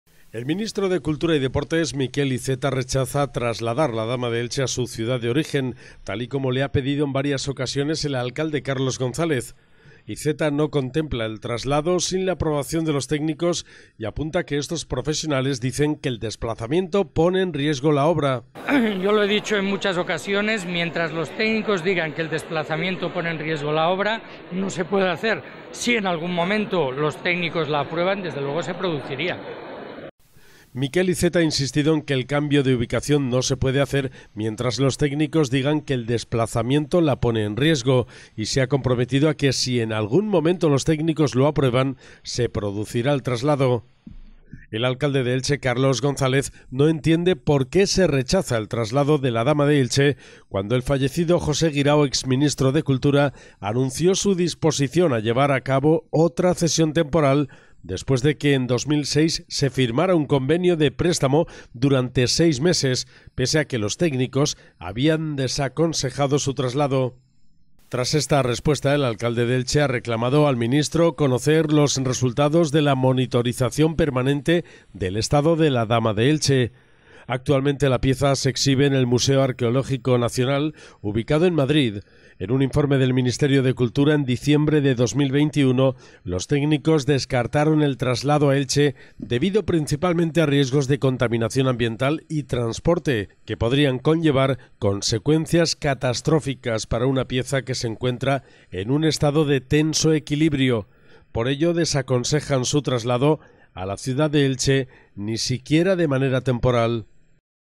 Iceta rechaza trasladar la Dama de Elche mientras los técnicos lo desaconsejen
El ministro de Cultura y Deporte, Miquel Iceta, rechaza trasladar la Dama de Elche a su ciudad de origen, tal y como le ha pedido en varias ocasiones el alcalde Carlos González. Iceta no contempla el traslado sin la aprobación de los técnicos, y apunta que estos profesionales dicen que "el desplazamiento pone en riesgo la obra".